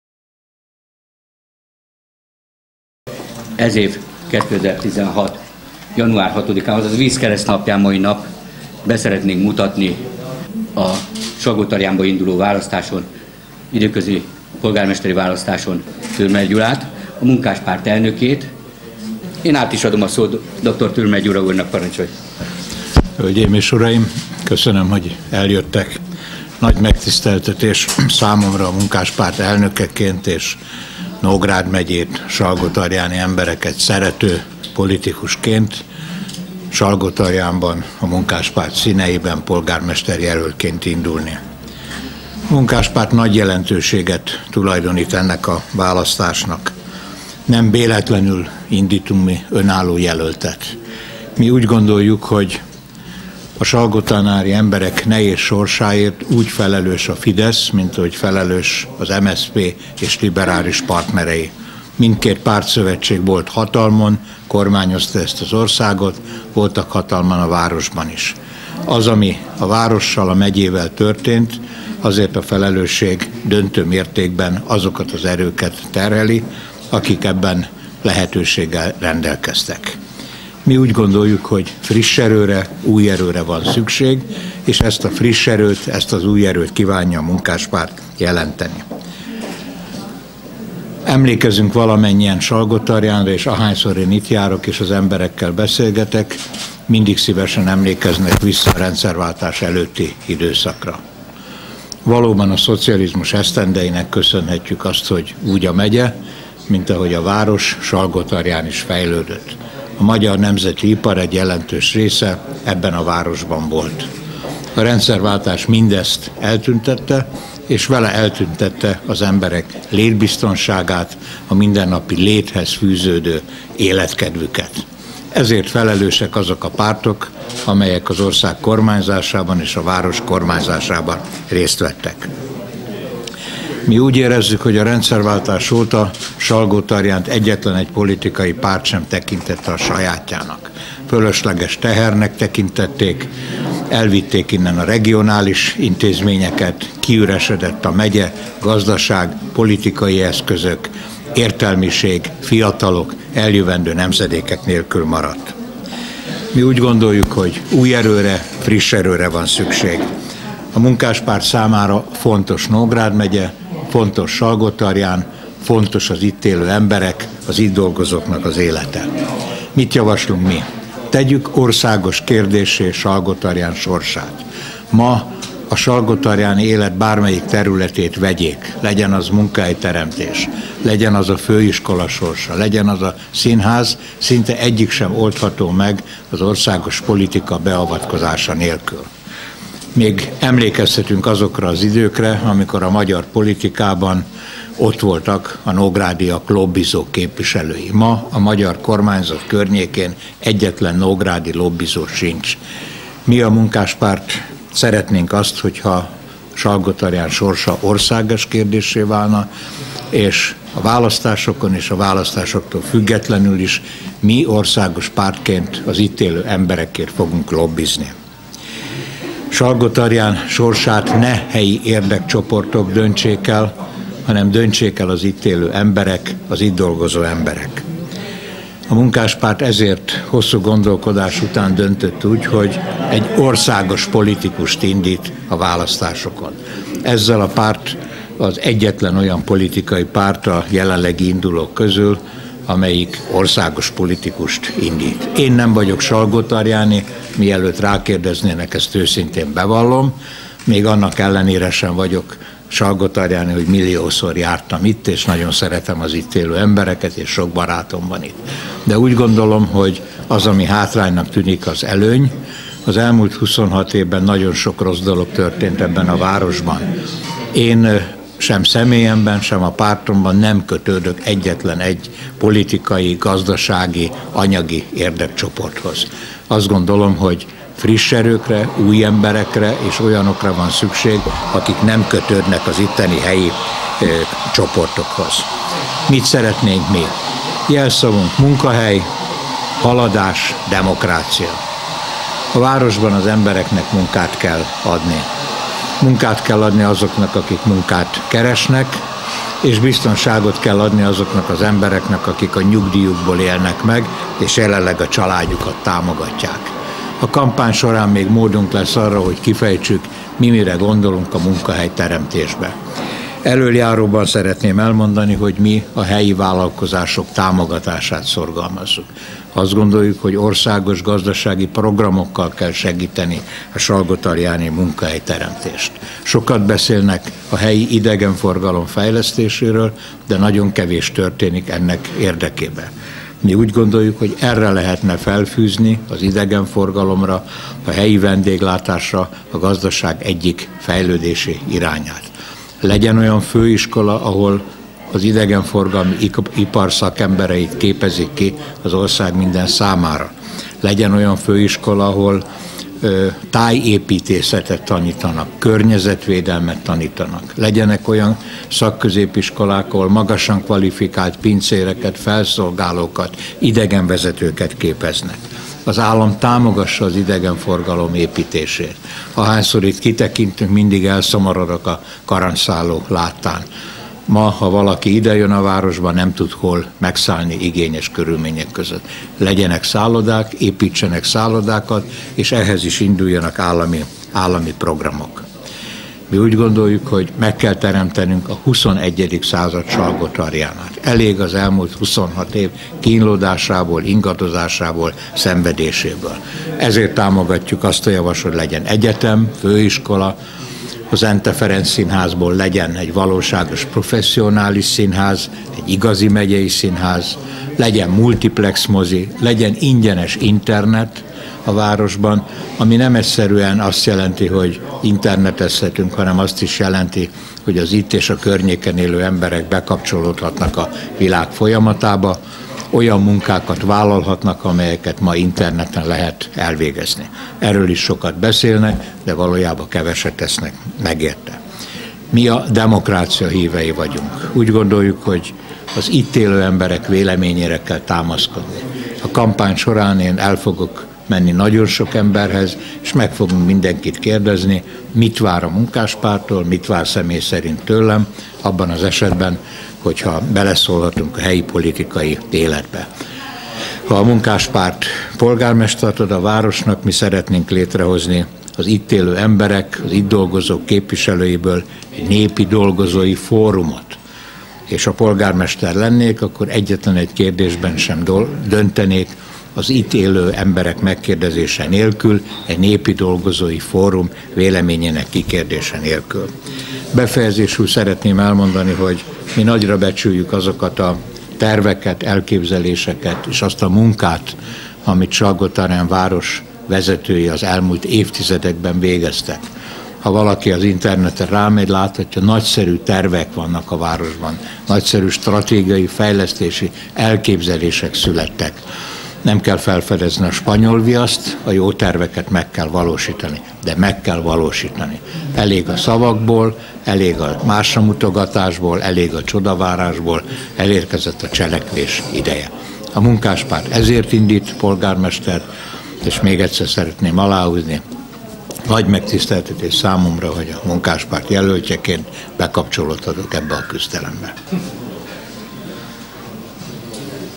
Salgótarján: Thürmer Gyula a Munkáspárt polgármester-jelöltje
Önálló jelöltet indít a Munkáspárt Salgótarjánban az időközi polgármester-választáson. A Munkáspárt színeiben Thürmer Gyula, a párt elnöke indul. – jelentette be Nagy Attila, a Munkáspárt Nógrád megyei elnöke január 6-án Salgótarjánban.

Thürmer Gyula kiemelte, hogy a Munkáspárt szerint a Fidesz és az MSZP, illetve liberális partnerei egyaránt felelősek azért, hogy Salgótarján lakossága nehéz helyzetbe került. A Fidesz is, az MSZP is volt kormányon, mindkettő vezette a várost is, még sem történt semmi.

Mindkét politikai erőt felelősség terheli azért, hogy az elmúlt évtizedekben engedték szétrombolni a város iparát, elkótyavetyélni az orosz piacot, eladni, privatizálni, szétlopni mindent, ami mozgatható volt.

Az évek során az is kiderült, hogy Salgótarján, és általában Nógrád megye sorsa senkit sem érdekelt a tőkés magyar kormányok közül. Még sokan emlékeznek arra, hogy a szocializmus idején erős "nógrádi lobbi" volt, maga Kádár János is oda figyelt a megyére, pedig az akkor sem volt nagyobb, mint ma. Az állam odafigyelése és a gazdaság fejlődése meglátszott a megye arcán. Salgótarján akkor lett kis bányászvárosból ígéretes jövőjű város.

Ugyanakkor tisztelettel adózunk a várost korábban vezető személyeknek és kollektíváknak. Salgótarján számára jó fejlesztési terveket készítettek. Meg kell őket valósitani!

Új erőre, a dolgozók valóságos érdekeinek képviseletére van szükség. A Munkáspárt számára fontos a Nógrádi megyei és salgótarjáni dolgozó emberek sorsa, és tenni akarunk érte!

Tegyük Salgótarján jövőjét országos kérdéssé! A város jövője attól függ, hogy mennyire tudjuk országos üggyé emelni. A Munkáspárt tesz egy fontos lépést, hiszen az egyedüli párt, amely országos politikust indít a választáson.

Salgótarján sorsát ne helyi érdekcsoportok, hanem a város lakói, az itt élő emberek döntsék el! Nem vagyok salgótarjáni, éppen ezért nem kötődők egyetlen helyi érdekcsoporthoz sem. Semmilyen felelősség sem terhei az elmúlt évtizedek városi ügyeiért. – mondta Thürmer Gyula.

Jelszavunk: Munkahely, haladás, demokrácia!

Legyen biztos munkája minden tarjáni embernek! A tönkretett ipar helyett új ipart kell teremteni. Valóságos idegenforgalmi ipart kell létrehozni, támogatva a helyi vállalkozásokat. Modern, magas színvonalú szállodákra van szükség! El kell kezdeni vendéglátók, felszolgálók, szakácsok magas szintű képzését. Az állam indítson nagy állami gazdasági programokat Salgótarján fejlesztésére, új munkahelyek teremtésére. Fontosnak tartjuk a kínai tőke bevonását. A Munkáspárt kész kapcsolatait ennek érdekében felhasználni.

Teremtsünk 21. századi Salgótarjánját! Egyetemet, saját színházat, multiplex mozit, ingyenes internetet minden itt lakónak!

Demokráciát! Hozzunk létre az itt élők, az itt dolgozók képviselőiből Népi-dolgozói fórumot. Megkérdezésük nélkül egyetlen nagy kérdésben se szülessen döntés!

Salgótarján, 2016. január 6.